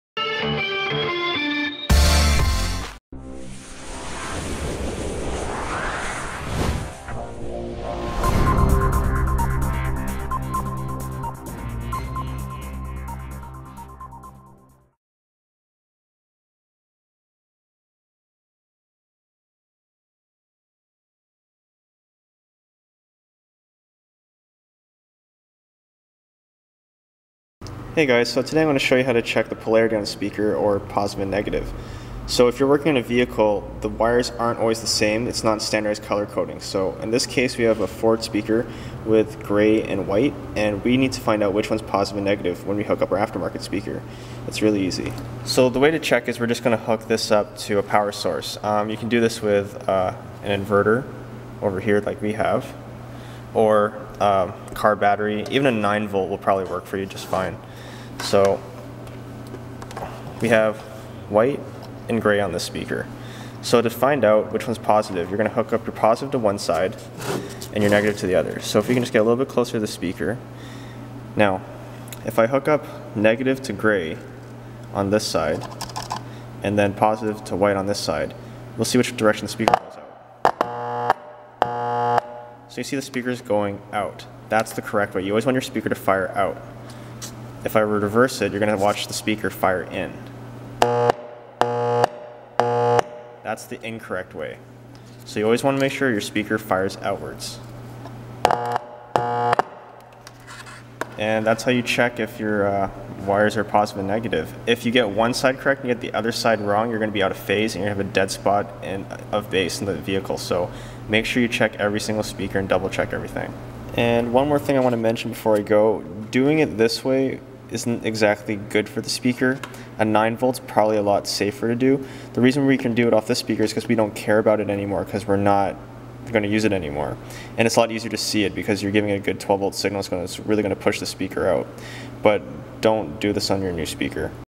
Checking for Positive & Negative on a Speaker (Speaker Polarity)
In this video I show you how to quickly and easily figure out which wire is positive and which wire is negative using the factory speaker as a guide.

| Please refer to our main Channel AnthonyJ350

Canada